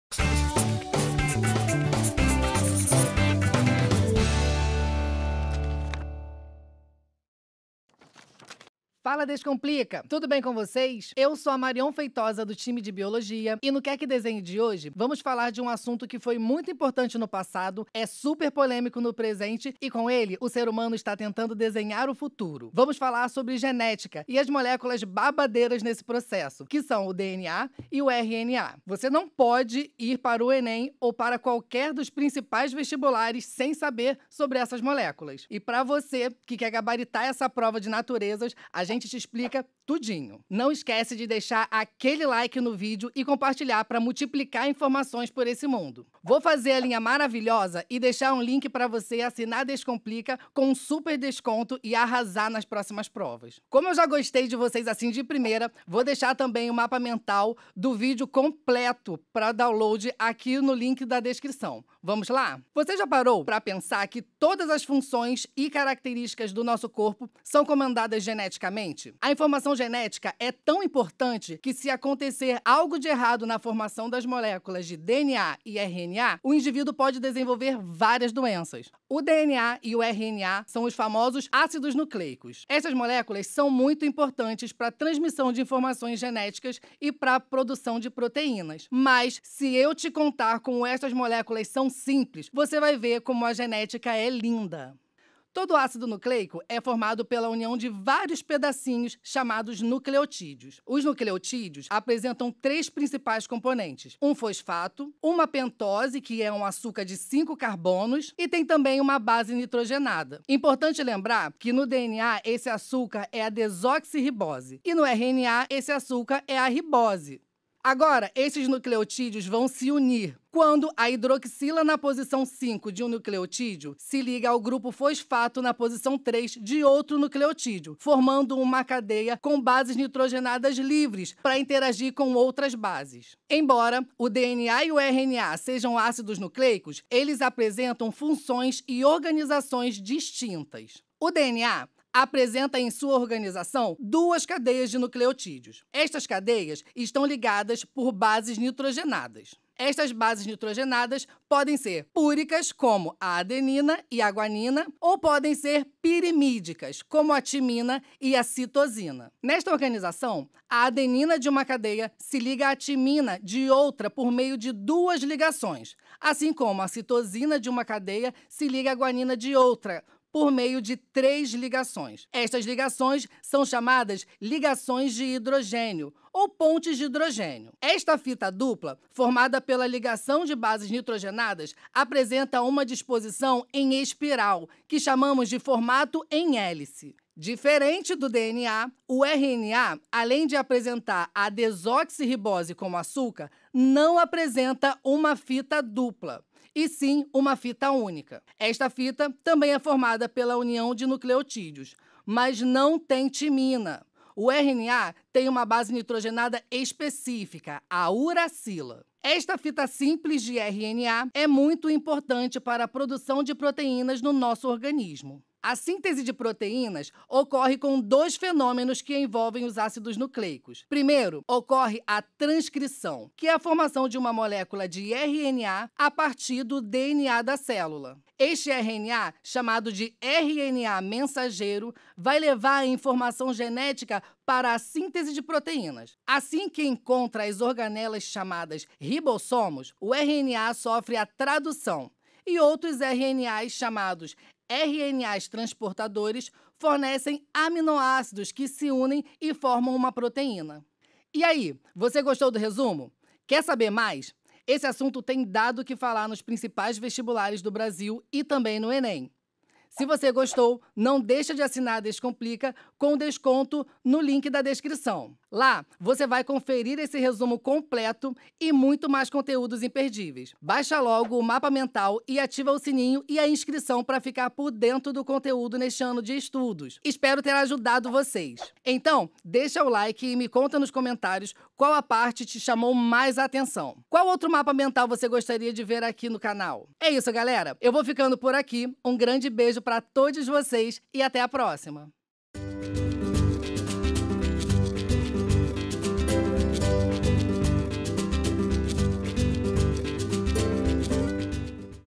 DNA E RNA | QUER QUE DESENHE | MAPA MENTAL
BORA FALAR SOBRE RNA E DNA?
*Errata: em 03:11, o correto é "apresenta a ribose como açúcar" e não "desoxirribose".
Já ouviu falar sobre o RNA e DNA? Essas duas siglas representam os blocos de construção da vida. O DNA é como um manual de instruções que guarda todas as informações genéticas de um organismo. Já o RNA desempenha um papel fundamental na leitura e execução dessas instruções. Neste vídeo, vamos desvendar os segredos do RNA e DNA, tudo pra te deixar pronto e você para arrasar nas questões de Biologia no vestibular. Não perde essa oportunidade e vem entender como essas moléculas governam nossa biologia e herança genética!
Continue com a gente!